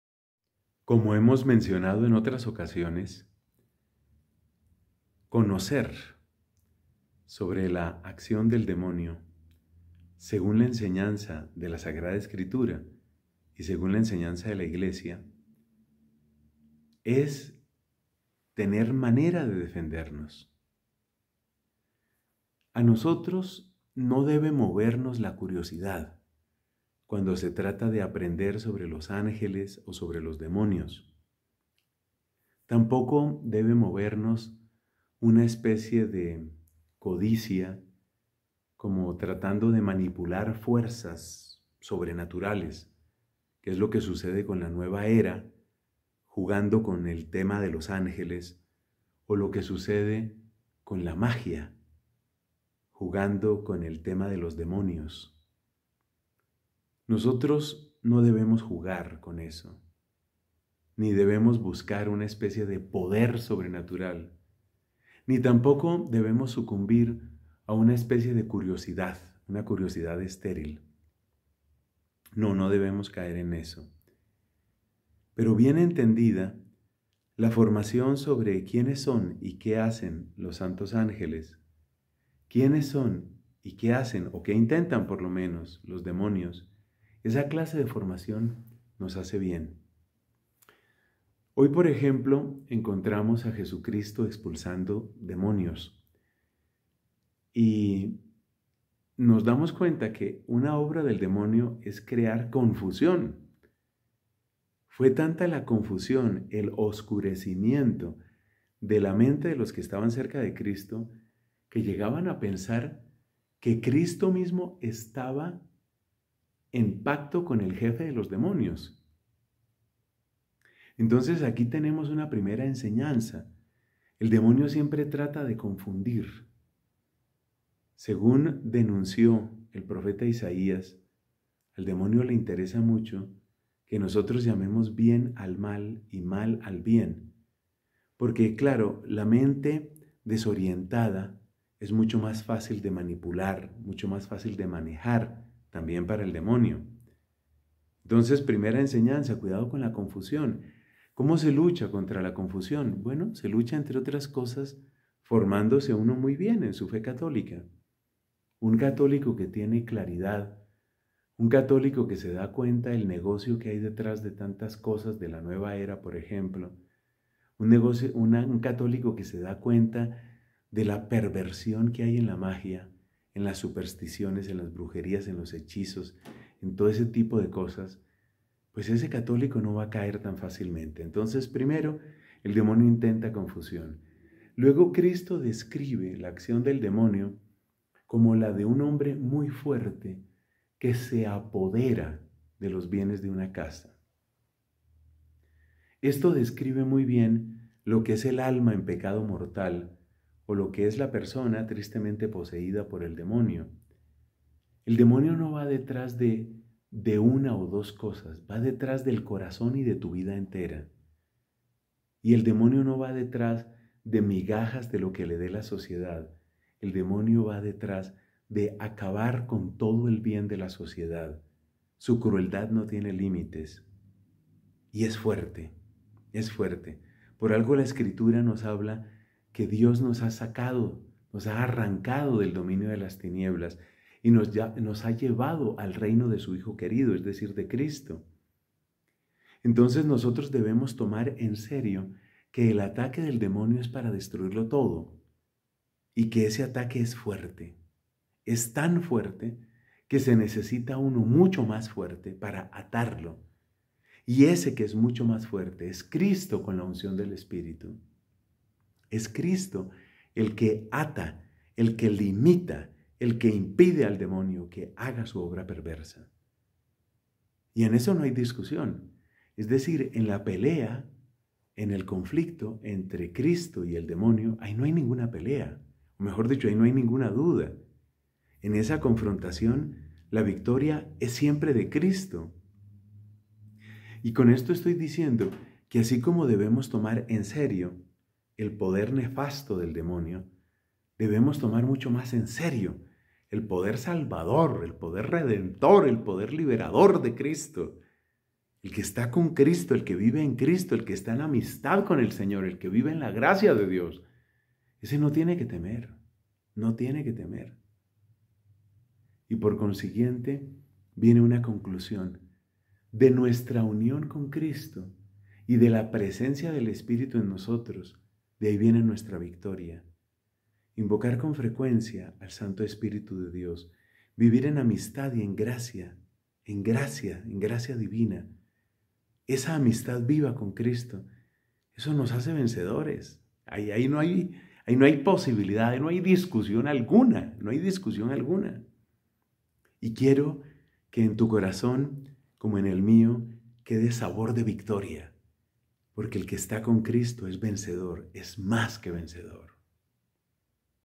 LA GRACIA 2022/01/24 Conocer el obrar del demonio para defendernos
Debemos conocer el obrar del demonio para defendernos: él trata de confundir; por eso debemos formarnos en nuestra fe y aunque él trate de destruirlo todo, nuestra victoria está en nuestra unión con Cristo y en la presencia del Espíritu en nosotros.